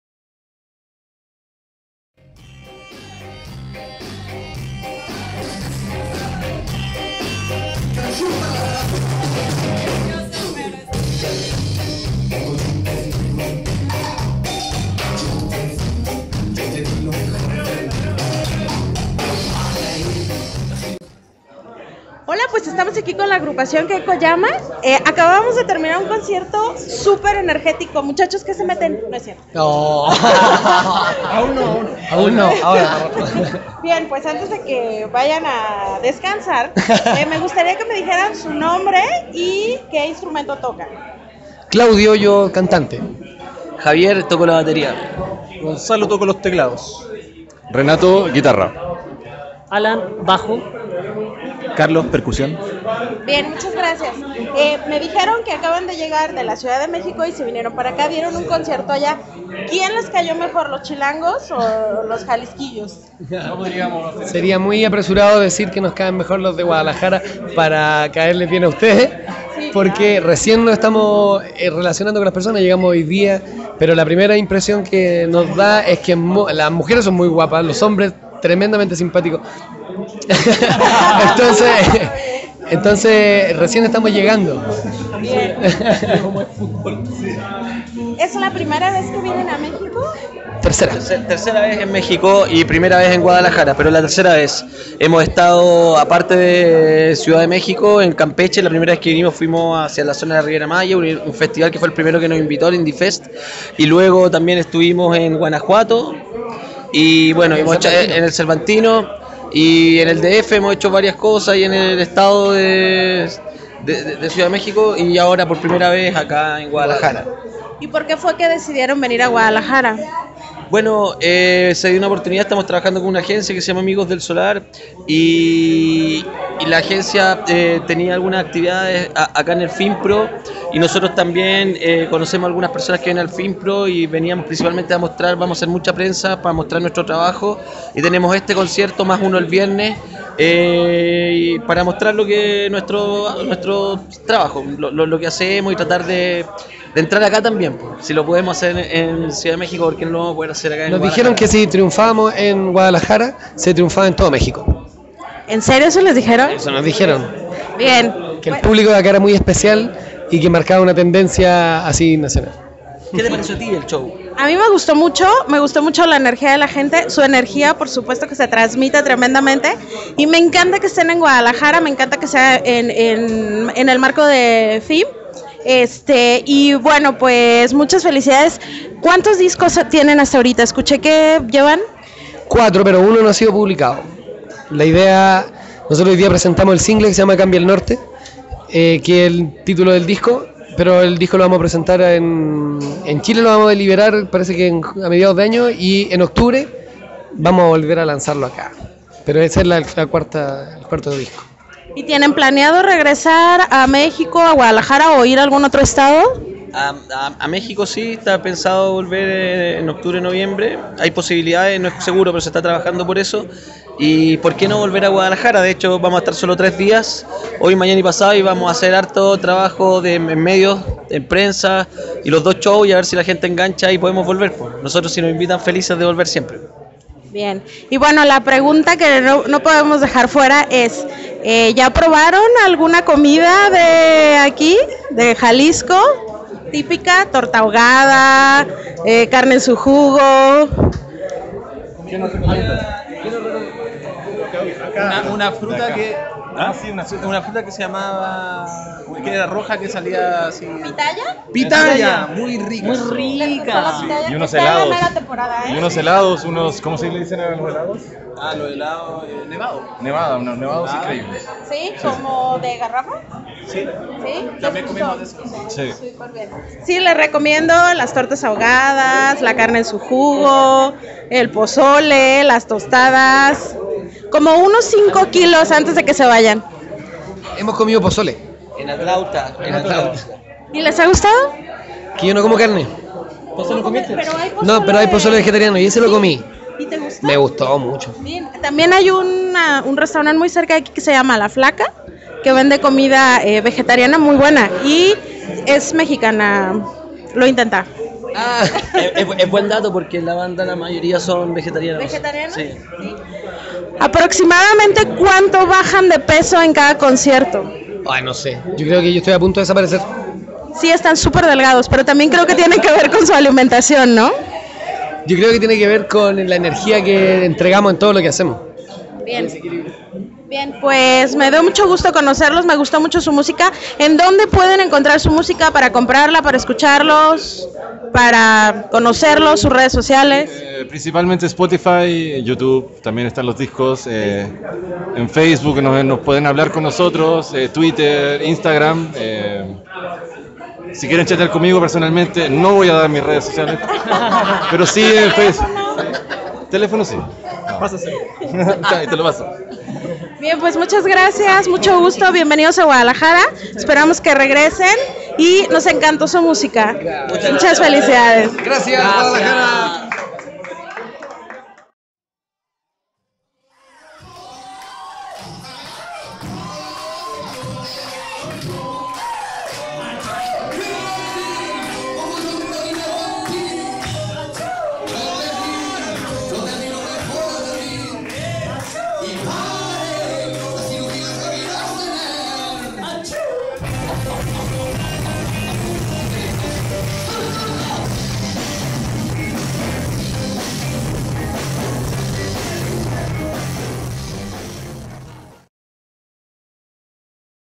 Entrevista con la banda KekoYoma
Les compartimos la entrevista con la banda chilena Keko Yoma que está de visita en Guadalajara para contagiar con su energía y su música a los tapatíos y que anoche hicieron bailar a todos en su presentación en Palíndromo